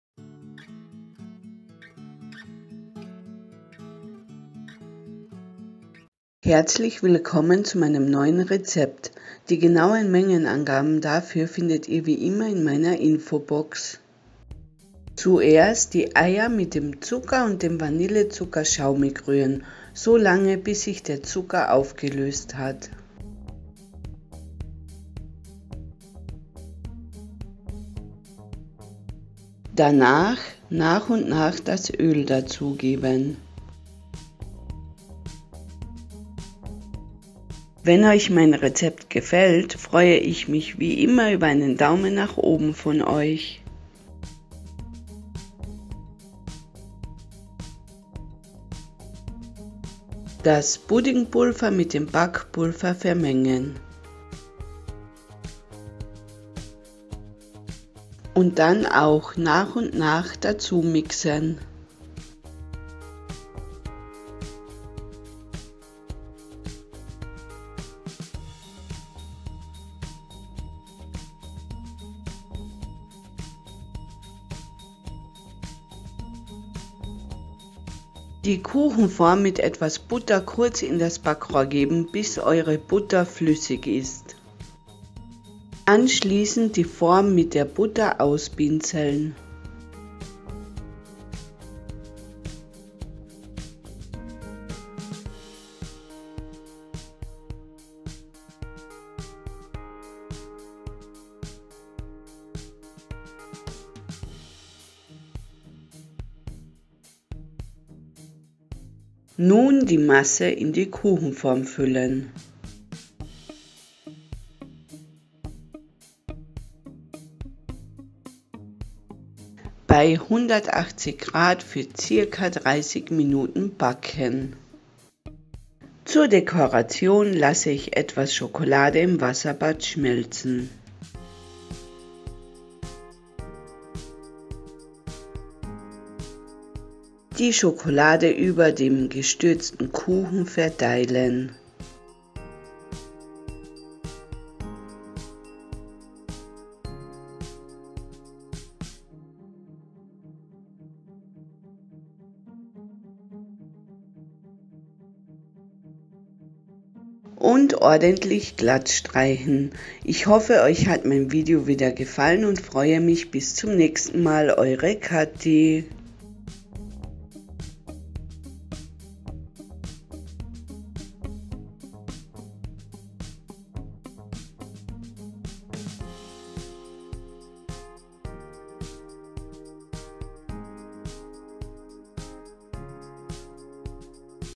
Puddingkuchen mit nur 6 Zutaten
NEU
Folgt mir auch auf Instagram:

Besucht mich gerne auf Facebook:

Meine Zutaten für eine kleine Kastenform

- 2 Pkg. Vanille Puddingpulver (oder eine andere Sorte)
- 70g Zucker
- 1 Pkg. Vanillezucker
- 2 Stk. glückliche Eier
- 3/4 Pkg. Backpulver
- 100ml Öl

Für die Form

- ca 30g Butter
- Mehl oder Semmelbrösel

Deko Blumen:
Wasserbad Topf:

Ich hoffe euch hat mein Video wieder gefallen.
Bis zum nächsten mal eure Kathii